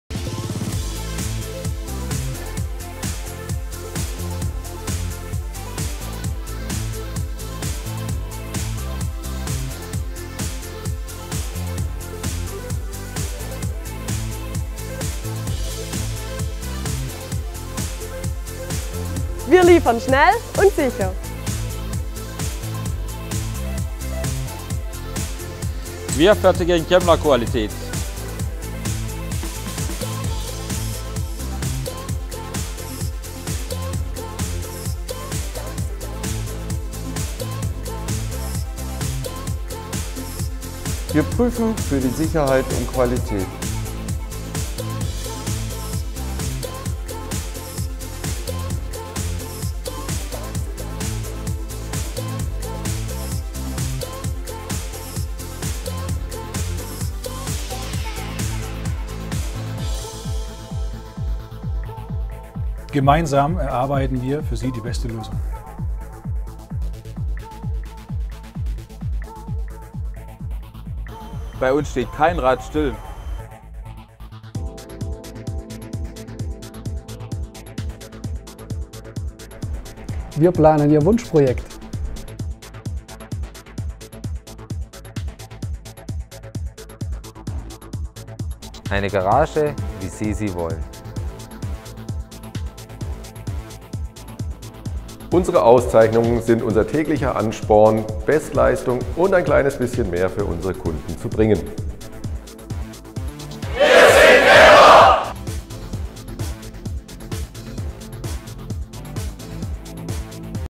Wir sind Kemmler
Dieser Unternehmensfilm lässt Sie einen Einblick in das Tätigkeitsumfeld des Unternehmen Kemmler Baustoffe in Hirschau werfen. Hierbei lassen sich Eindrücke aus den Bereichen der Auftragsbearbeitung, der Planung und Konzeption sowie der Produktion gewinnen.